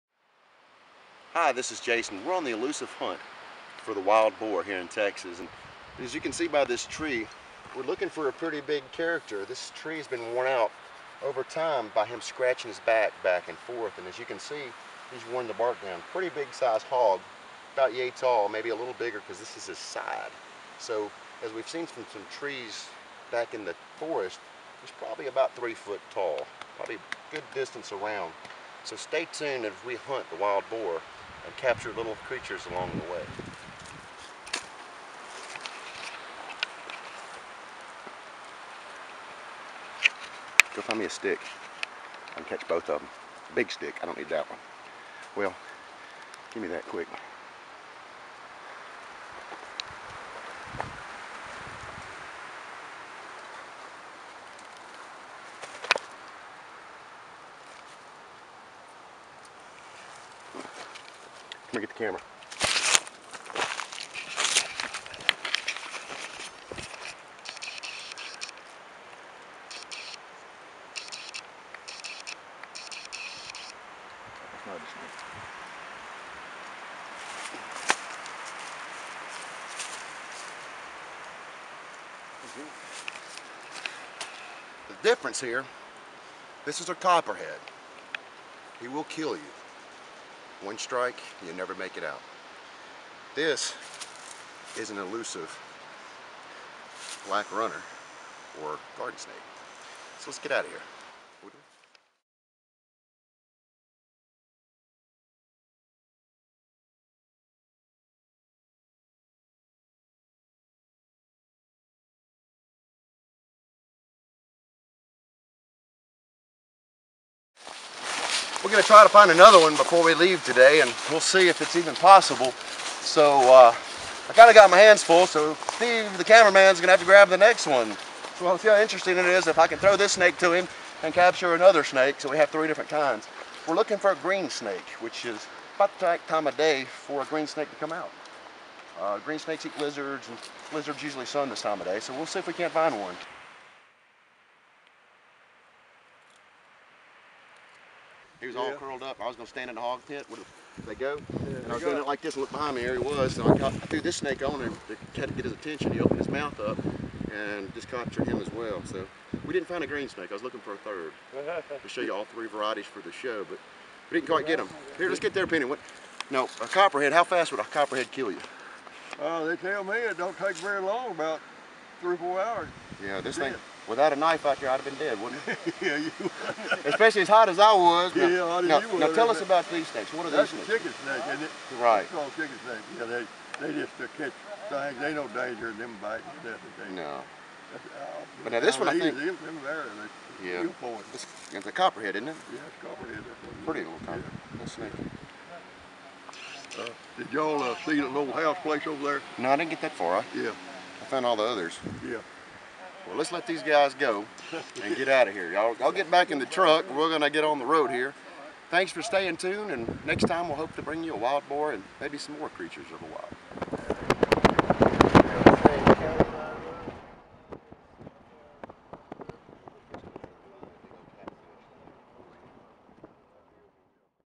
Snake bite
Watch as I catch two snakes at the same time... get ready to JUMP outta your skin... this was quit risky...